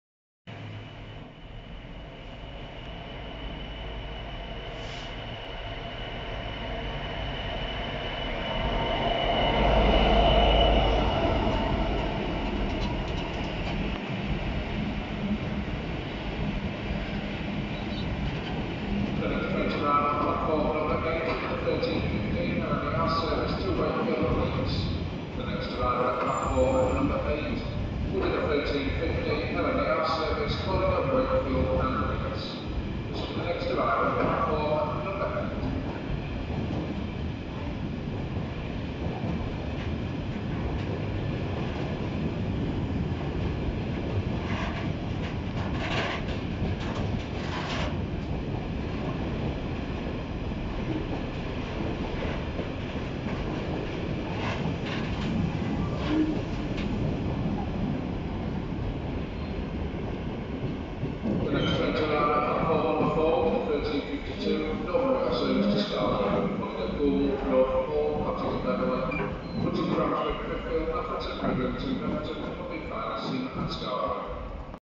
Class 66 Quad Passes Through Doncaster Platform One
Returning from Tyne Yard, 66712 "Peterborough Power Signal Box", 66707 "Sir Sam Fay", 66789 "British Rail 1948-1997" and 66751 "Inspiration Delivered - Hitachi Rail Europe" tow engineering train wagons back to Doncaster Decoy.